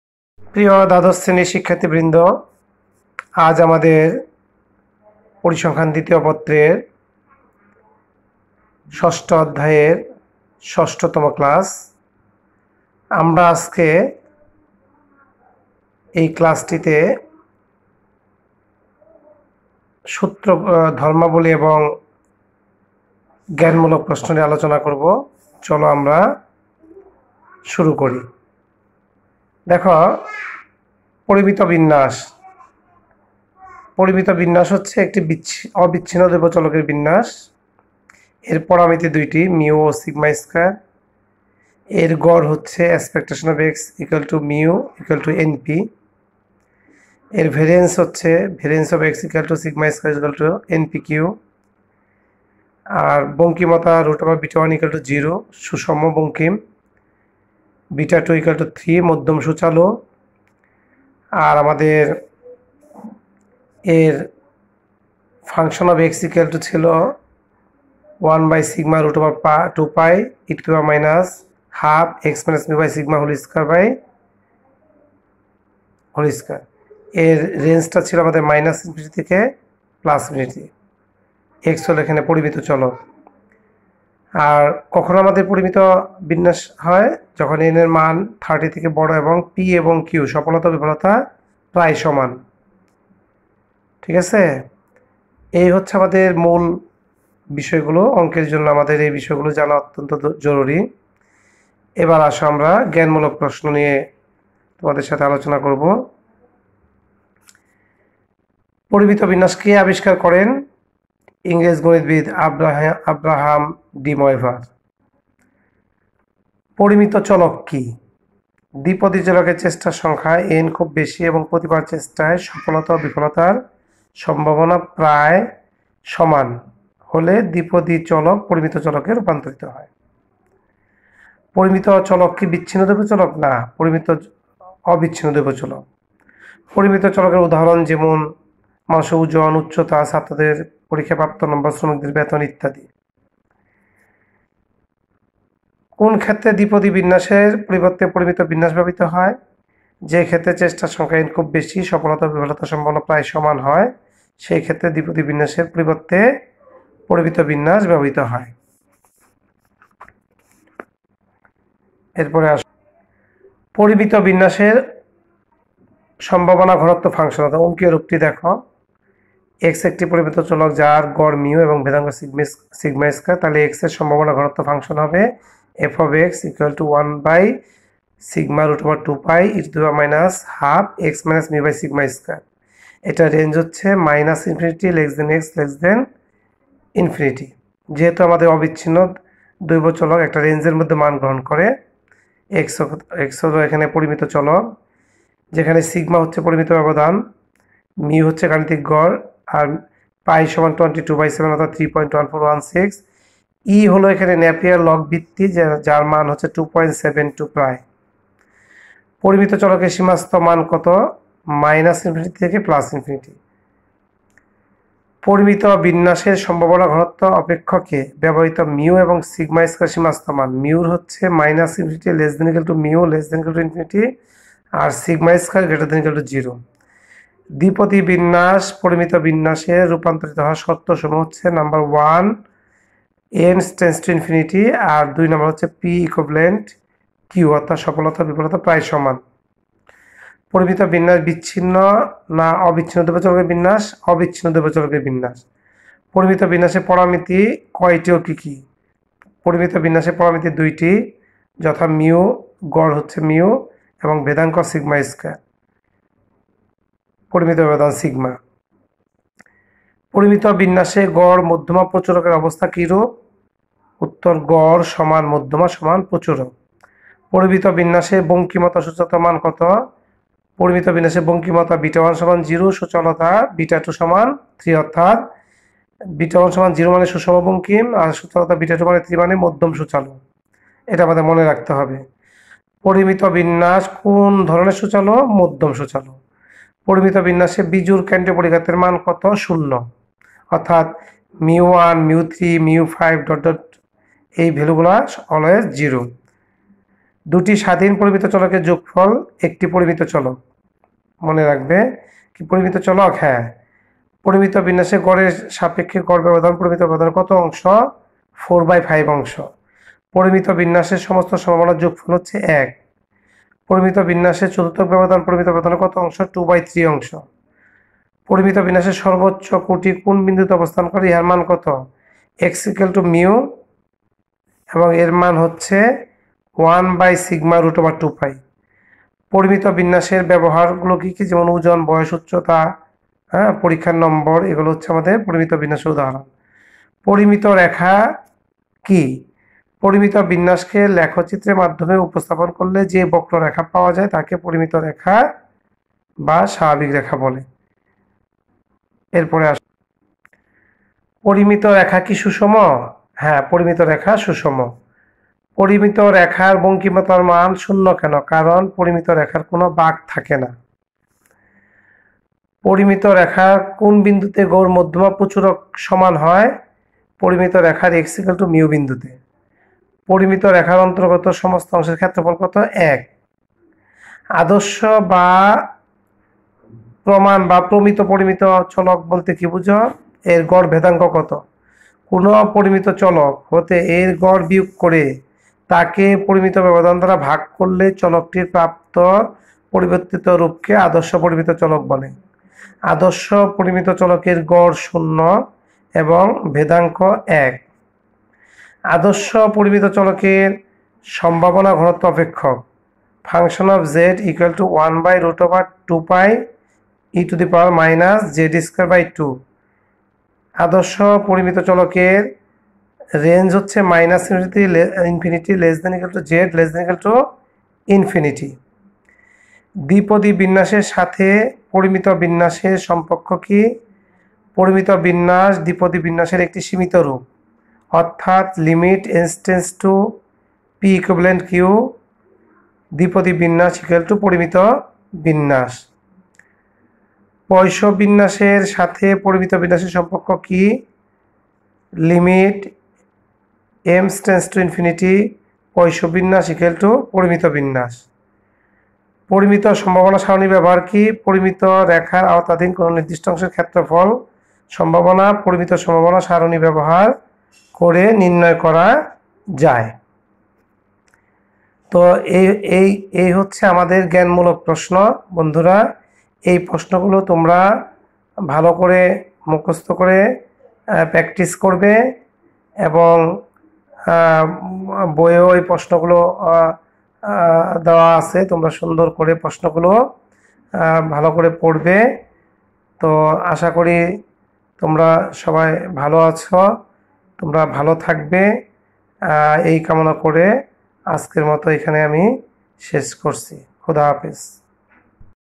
4 May 2020
Class:XII
Subject : Statistics 2nd paper
Chapter No : 6
Lesson : পরিমিত বিন্যাস
 Lecture No : 6
Teacher's Name : Raghu Nath Banik